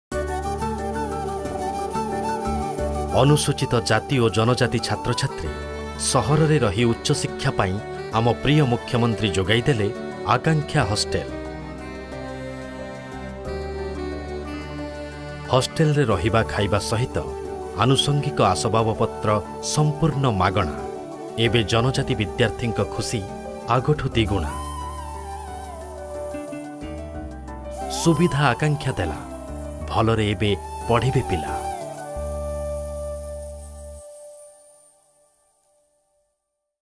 Akankshya - Hostels for Tribal Students
Akankshya Yojana is a social welfare programme initiated by the Government of Odisha for uplifting and nurturing the inherent talent of backward classes, and give them education. The Akankshya Yojana provides hostels to Dalit and tribal students pursuing higher education in various fields.